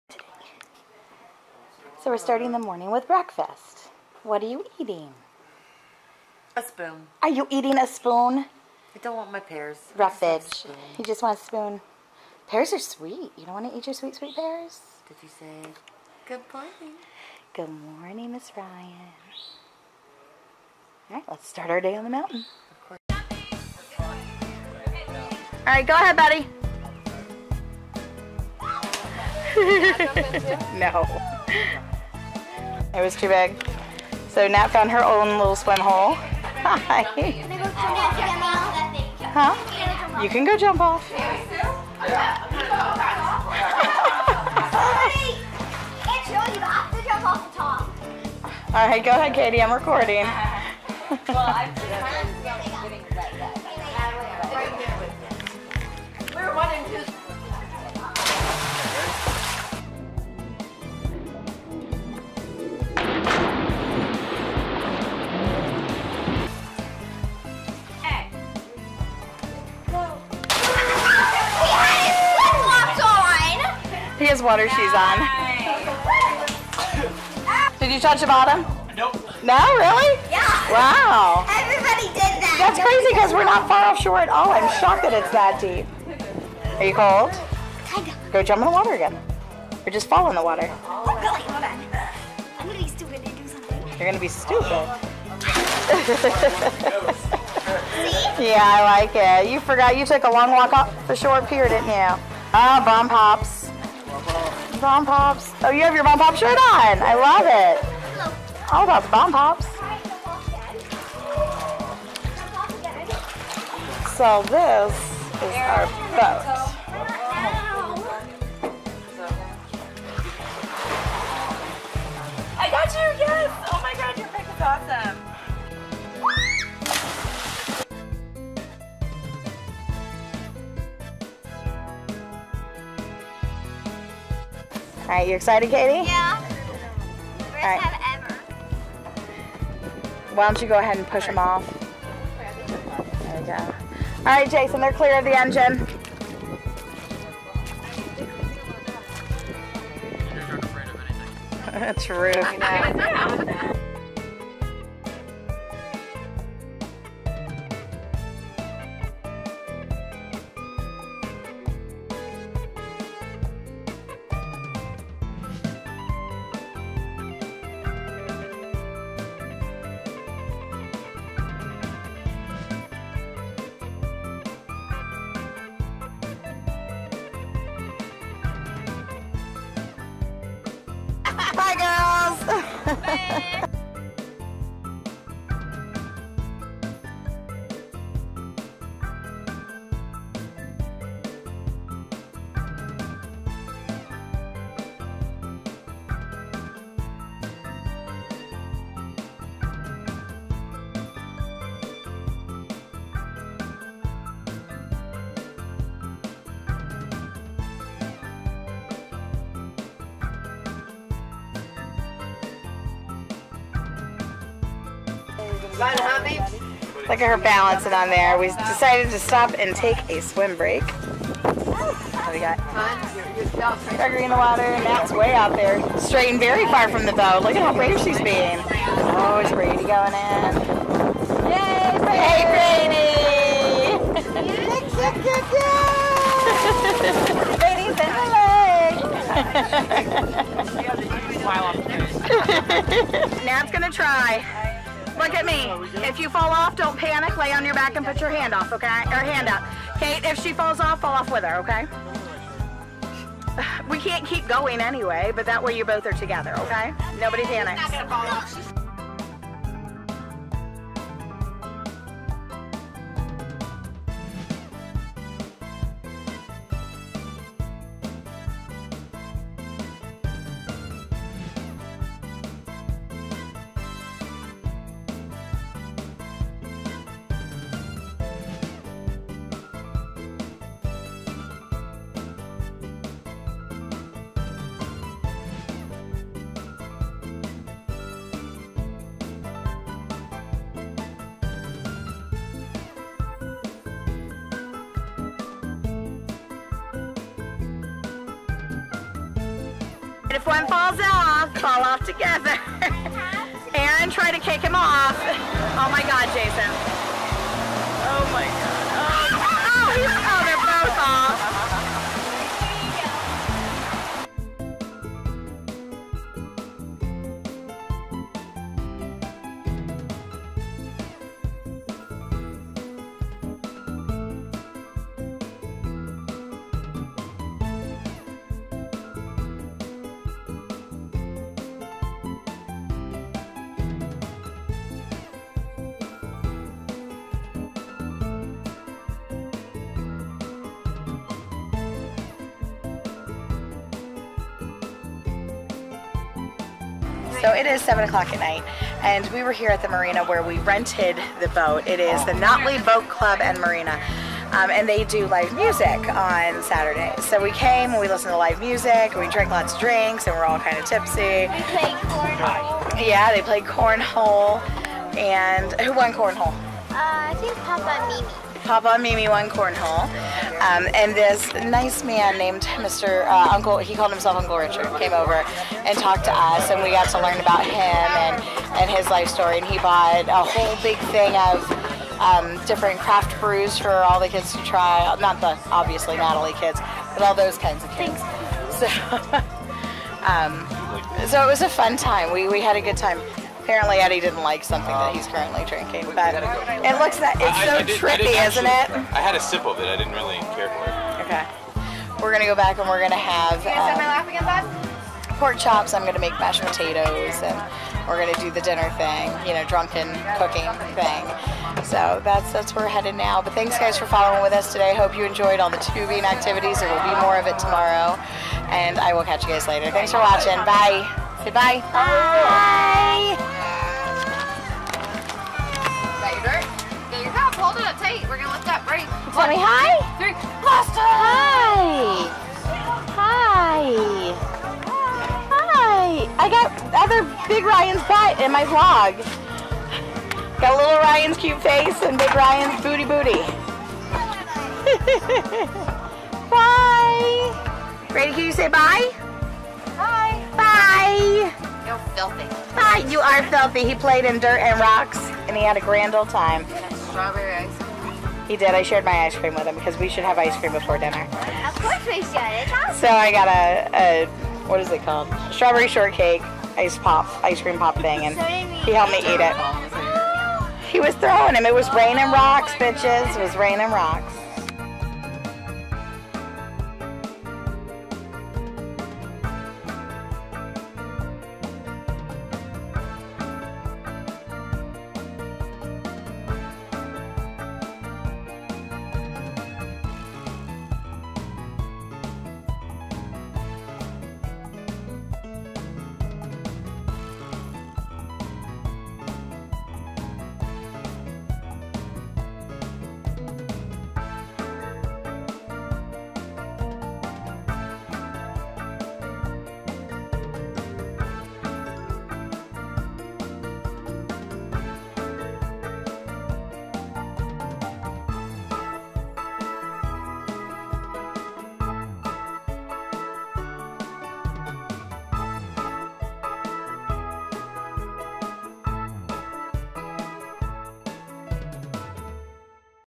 Tubing On Lake Nottely | Vlog #16
Today we go pick up the boat! We enjoy lots of tubing and swimming and jumping off the second story of the dock! Lots of fun!

My Camera